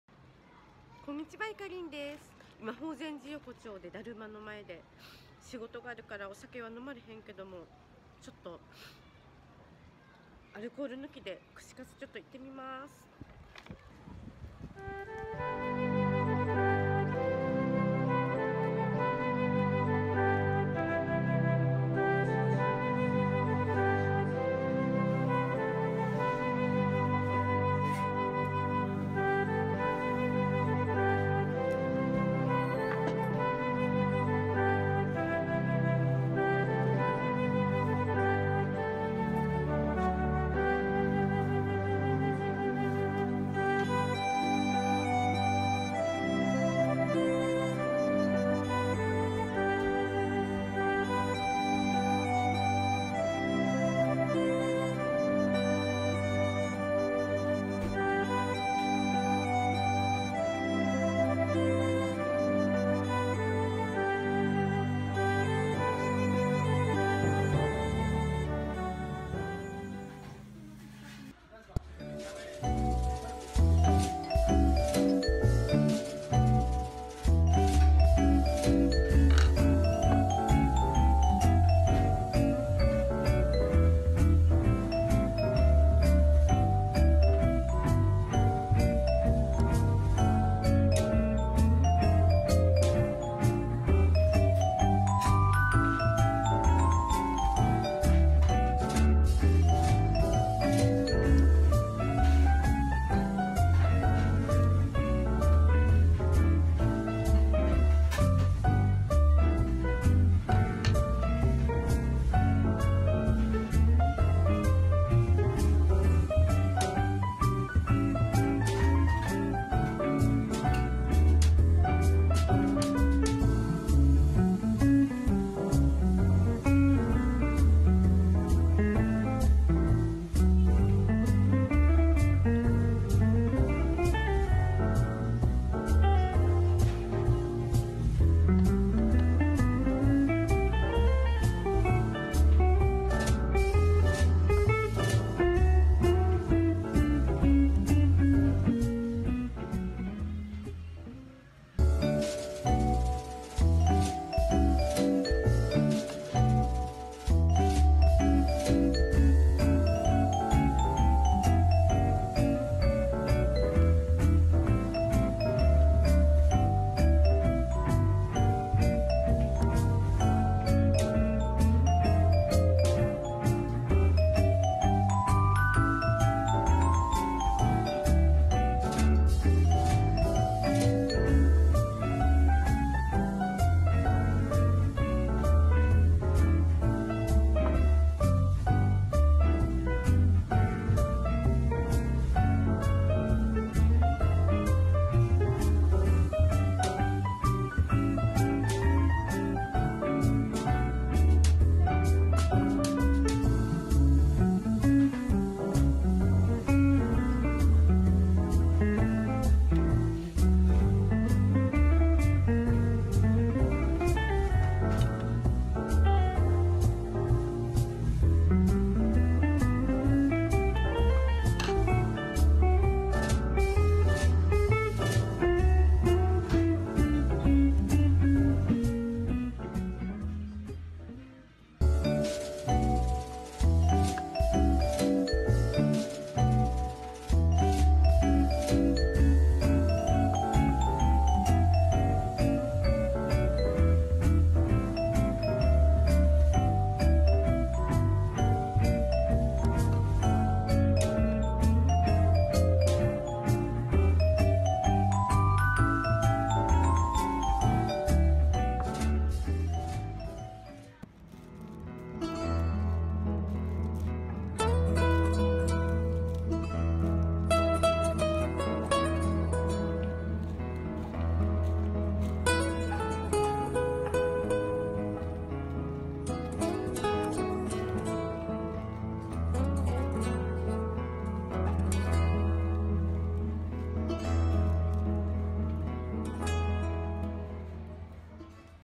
アラフォー独身女の1人串カツ。ノンアルコールビールで乾杯♪
ミナミの用事があり、どうしても串カツが食べたくなりました！

だるま限定で(笑)

仕事があるので一人飲み初めてのノンアルコールで頂きました（泣）

やっぱ、だるまの串カツ衣が薄くてサクサクで美味しかったなぁ♪